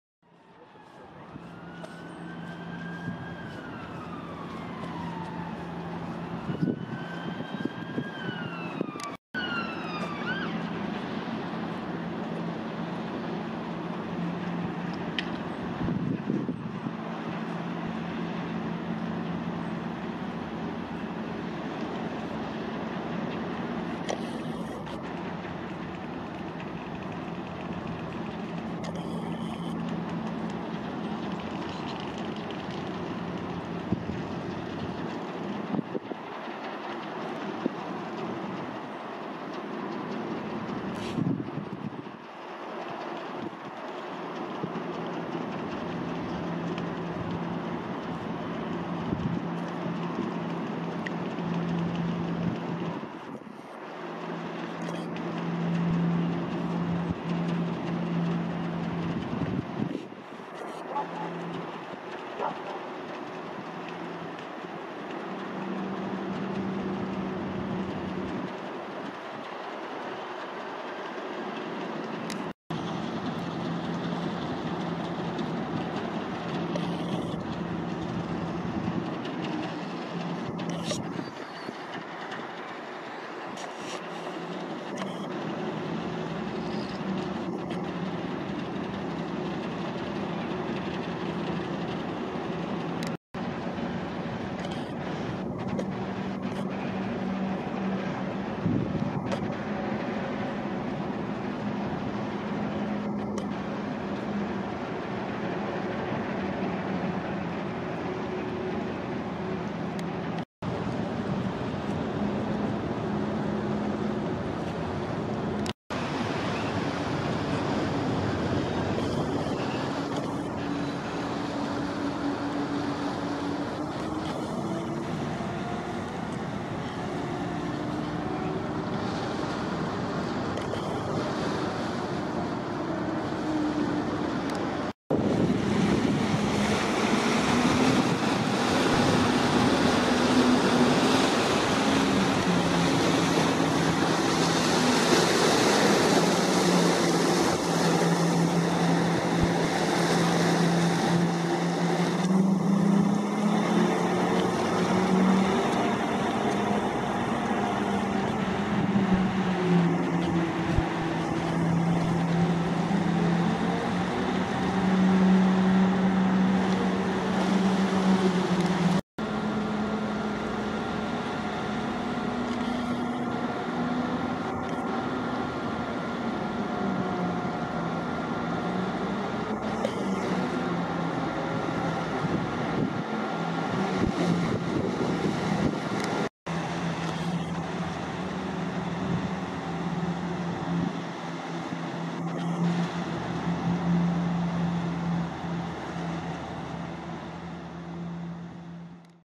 Hebridean Princess inbound Greenock for Winter layup
Hebridean Princess a small luxury cruise ship that mainly sails around the west coast of Scotland in the summer months.